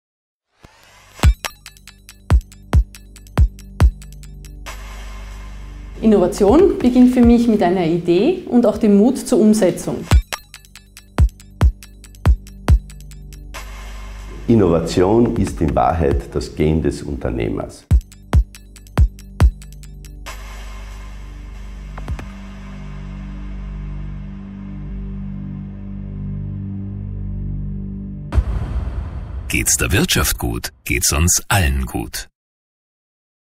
WK-Innovation
Präsident Franz Pacher und Expertin DI Elisabeth Hauer über Innovation.